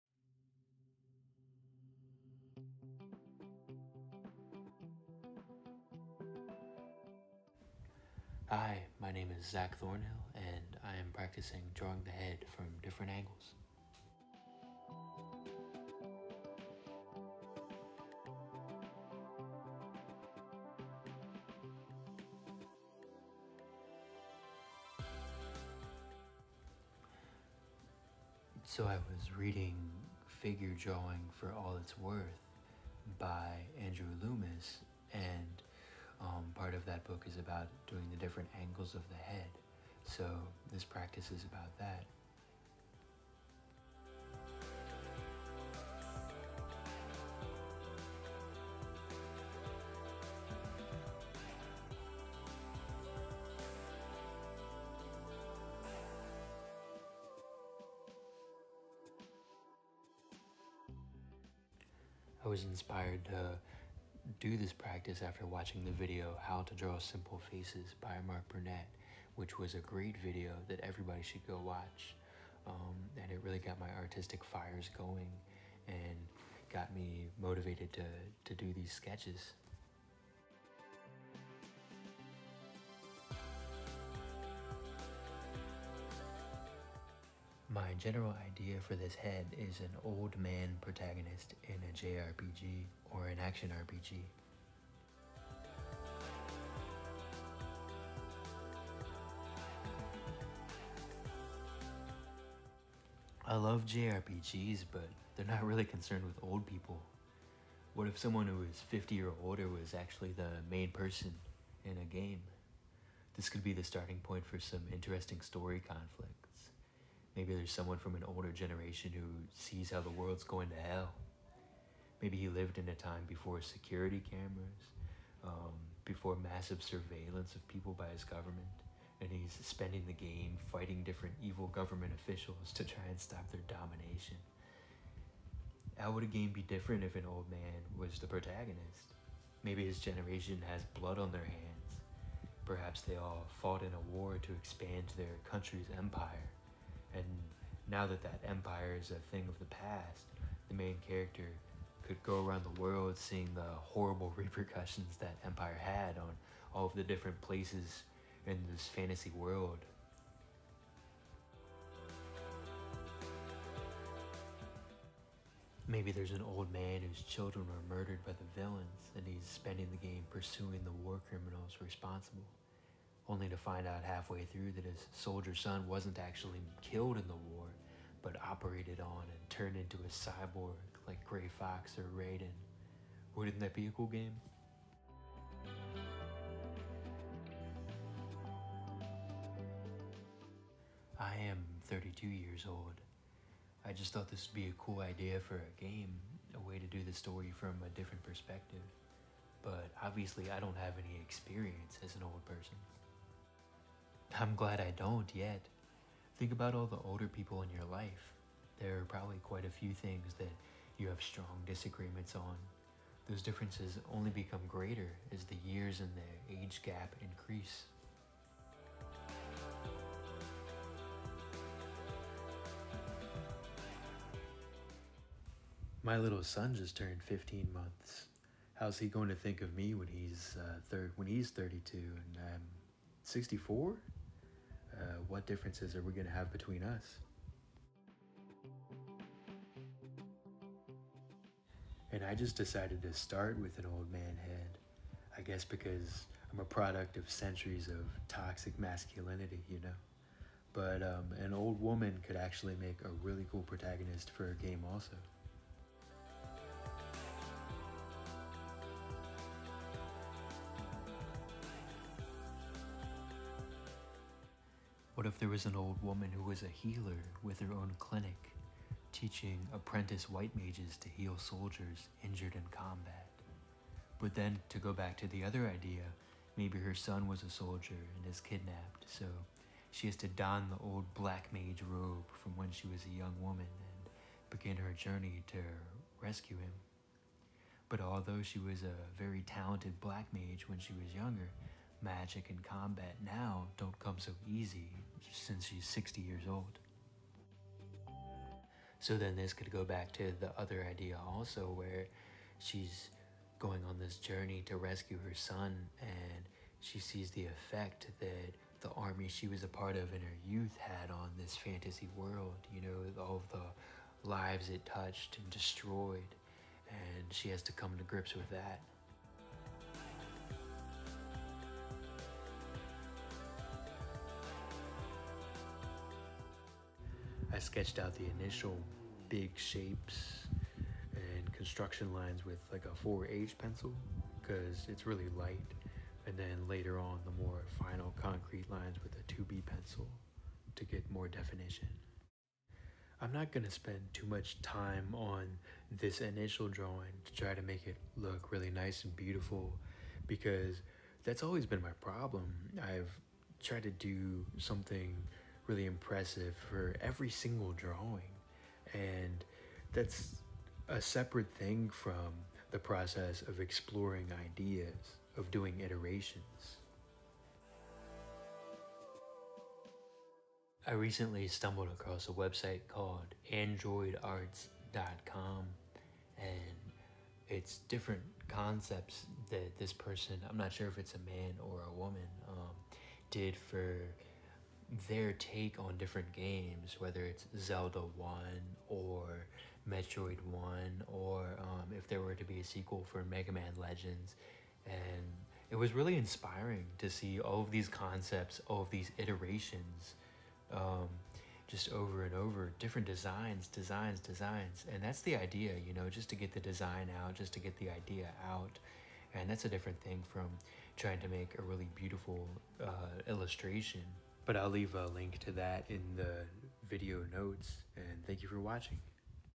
JRPG Design Practice
Join me as I transfer ideas from my imagination to the sketchbook page.

(Sorry the camera is shaky in the beginning, I am looking for a solution to get a good filming set up going).

Who are your favorite JRPG characters? What are your 3 favorite games? (Don’t have to be RPGs)

Thank you for joining me here on the internet 😁